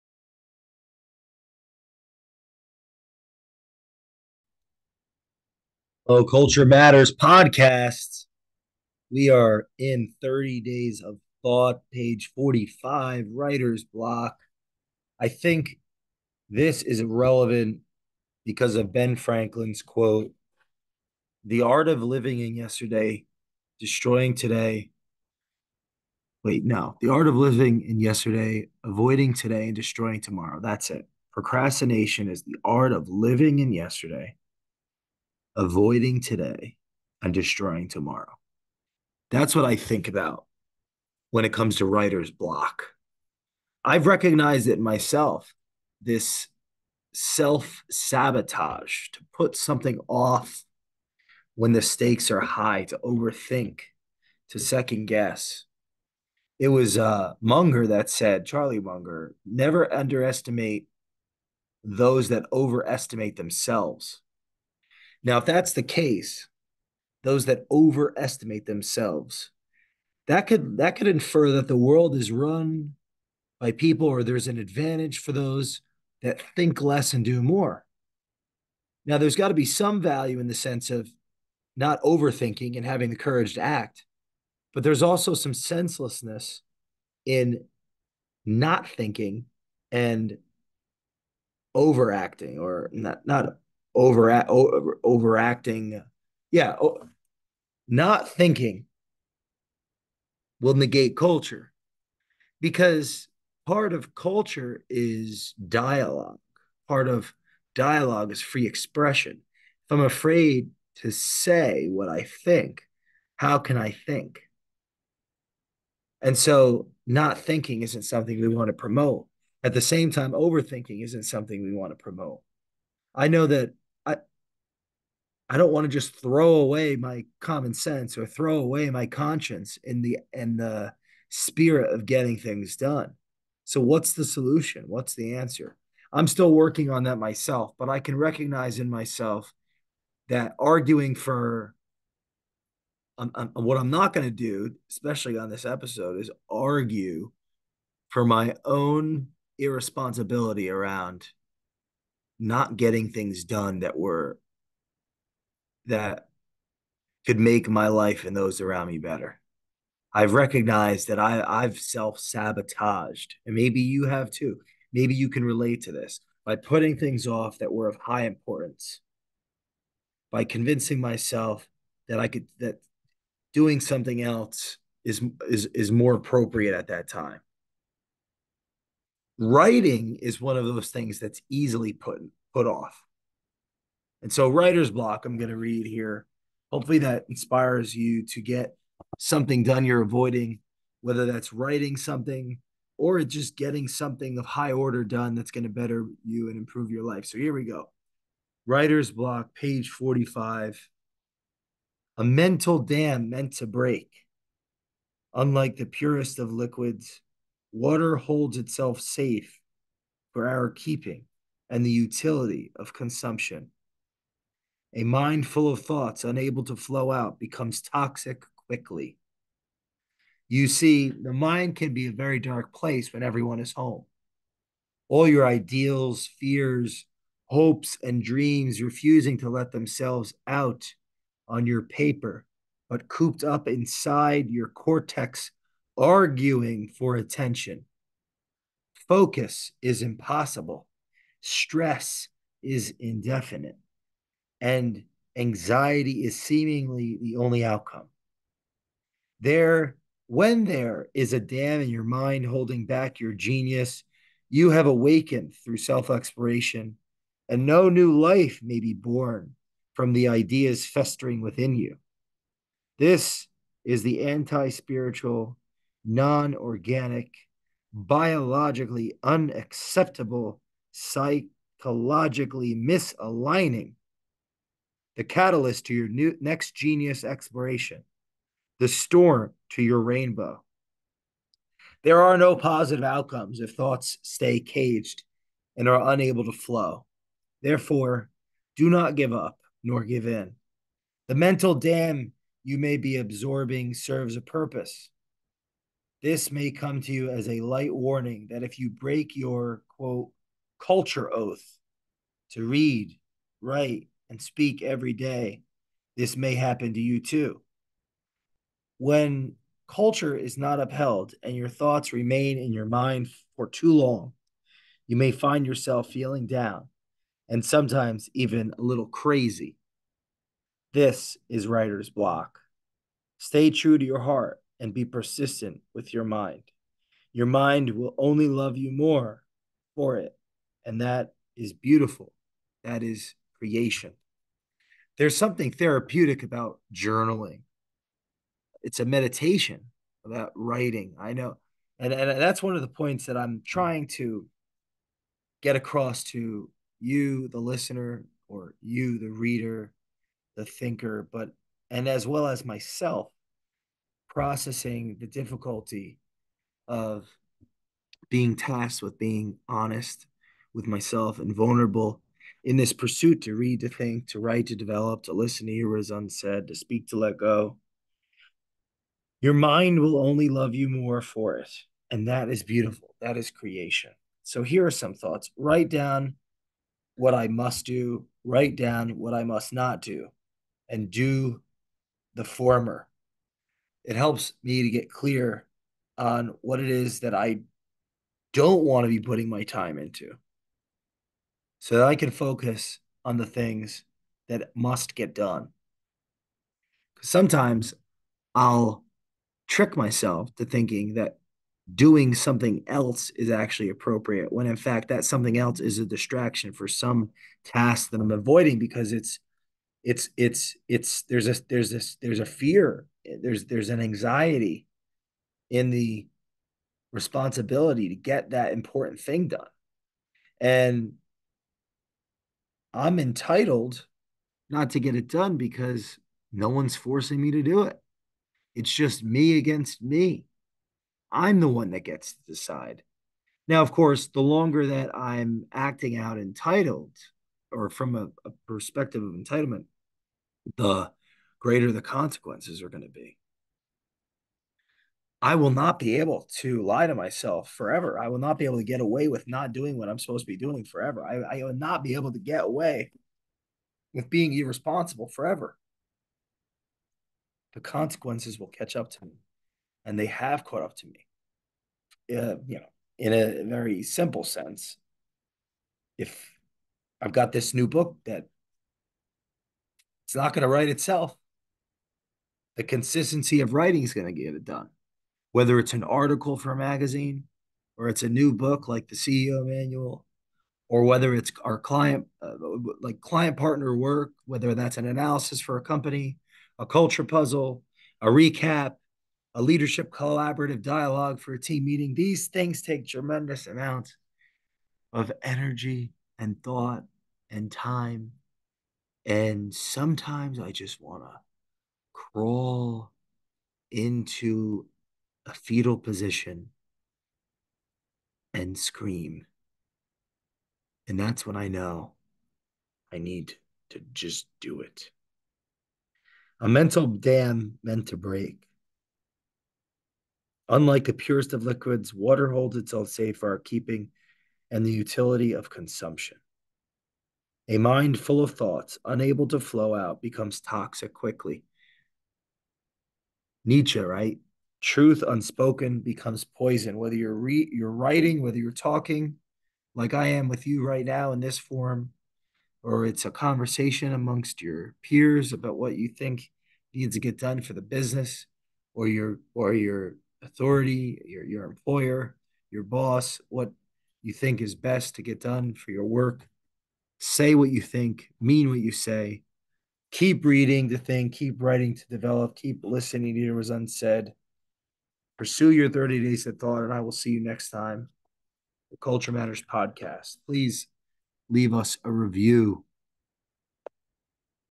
Thirty Days of Thought: Writer's Block, Episode 602
On this episode of The Culture Matters Podcast, it is time for part 17 of our ongoing series where our very own Jay Doran discusses his book, Thirty Days of Thought, and goes through the process right alongside our listeners.  Today, Jay is talking about the scourge of the creative mind, writer's block.  What it can mean for your culture, your motivation, and your overall well-being just goes to show what a menace it can be.  Have no fear for it can be overcome and that's exactly what we're talking about on this episode of The Culture Matters Podcast.